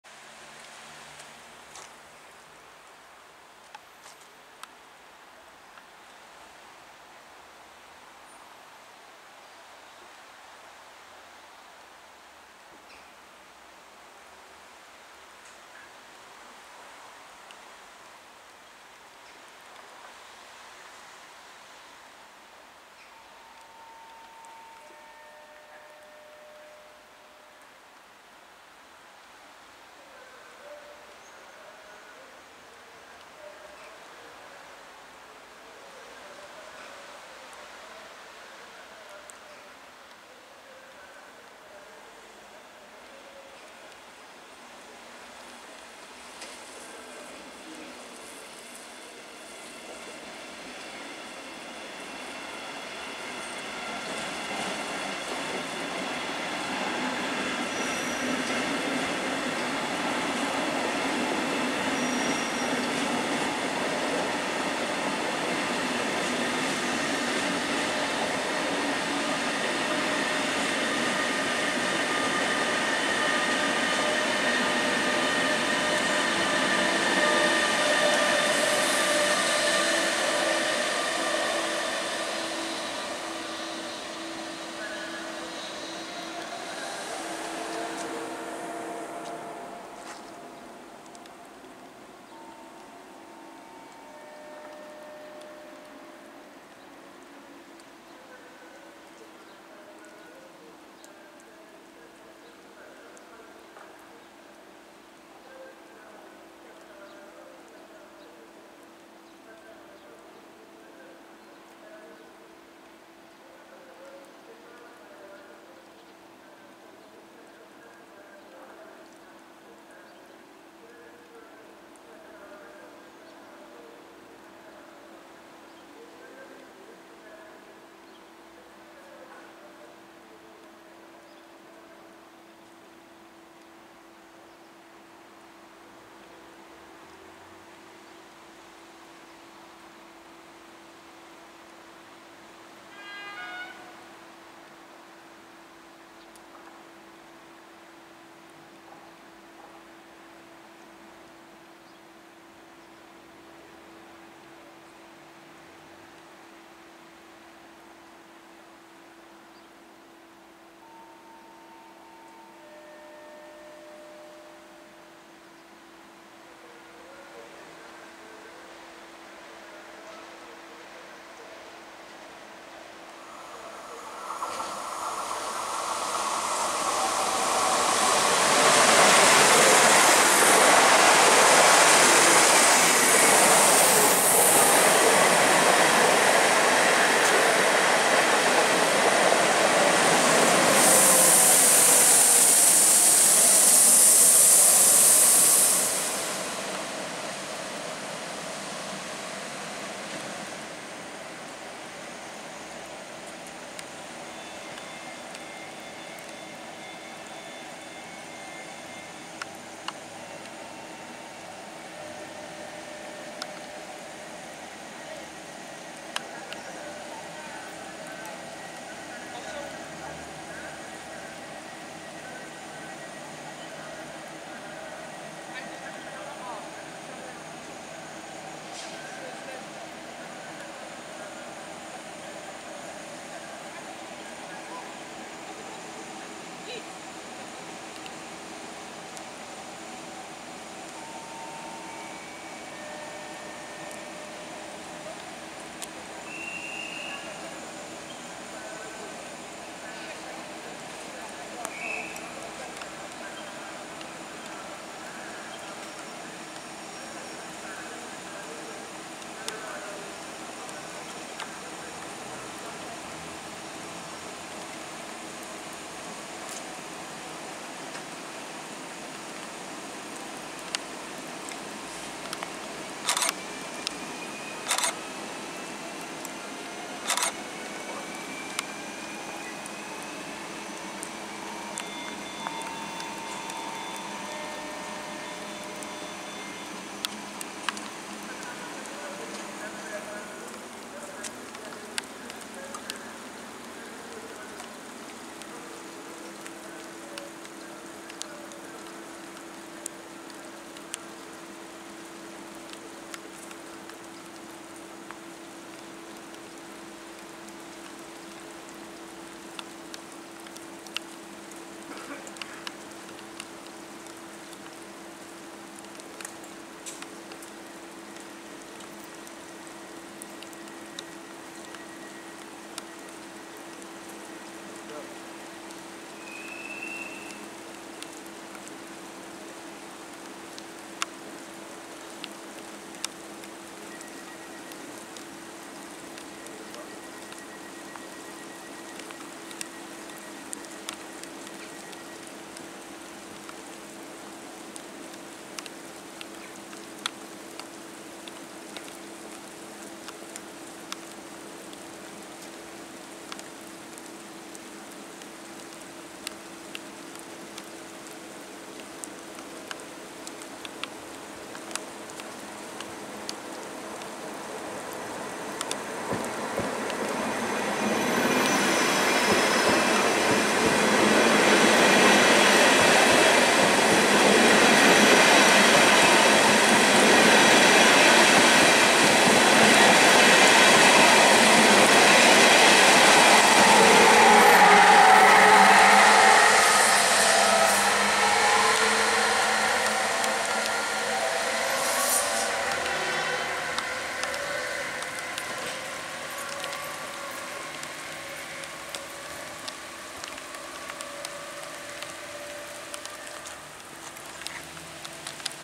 Warto było zmoknąć :D
Dla takiego ujęcia po prostu warto było :) Wstawiający się w perony EU47-010 "Hetman Jan Zamoyski" z osobowym do Siedlec oraz wjazd  duetu Impulsów 45We-008/002 z osobowym do W-wy Wsch. a na dokładkę wjazd i odjazd 111Eb-002 z "Wiedenką" do Skierniewic. Data nagrania: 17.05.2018